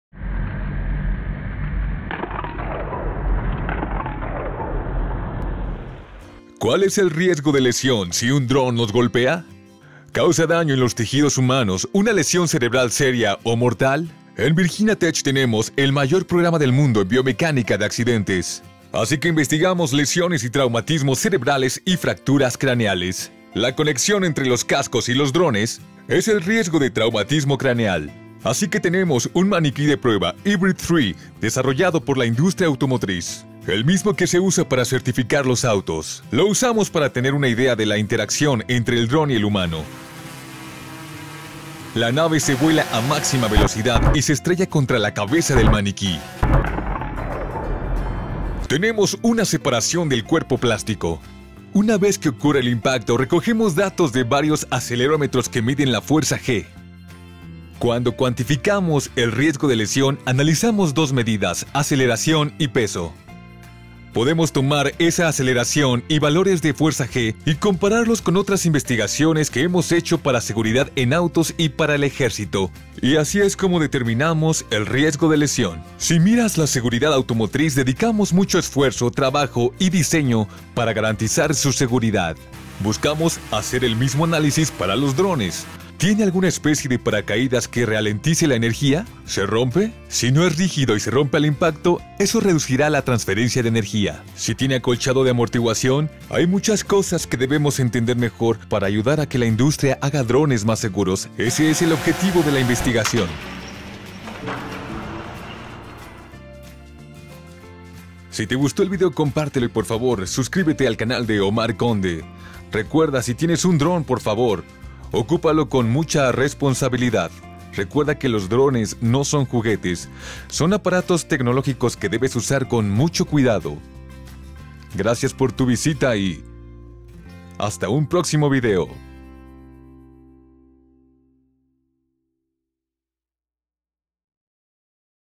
Haciendo Locución a un video
En este video hago el trabajo de locutor y explico lo que sucede si un Drone te golpea.

Escríbe tus opiniones en los comentarios y danos sugerencias de lo que te gustaría ver en este canal.